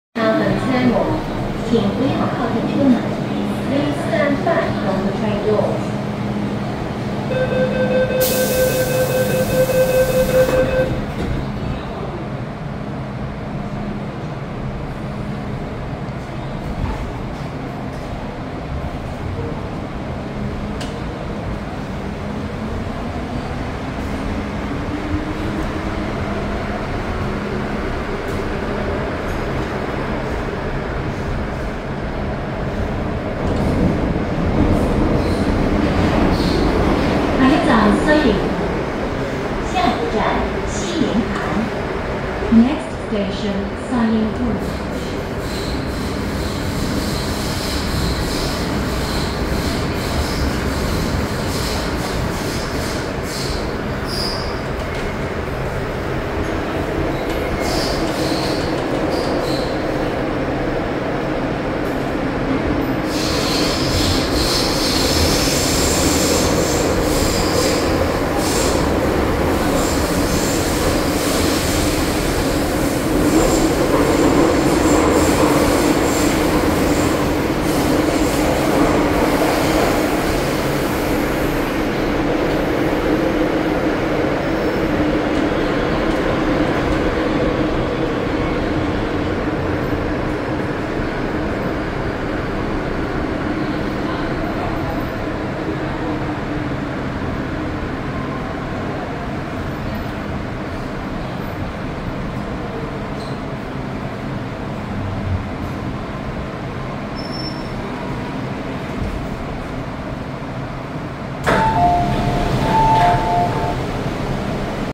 [홍콩철도풍경] 홍콩 MTR 홍콩섬선 케네디타운행 열차 셩완→사이잉푼 주행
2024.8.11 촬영

메트로카멜 직류형 전동차입니다 아직까지 췬완선에는 모든 열차가 운행하고 있다고 하네요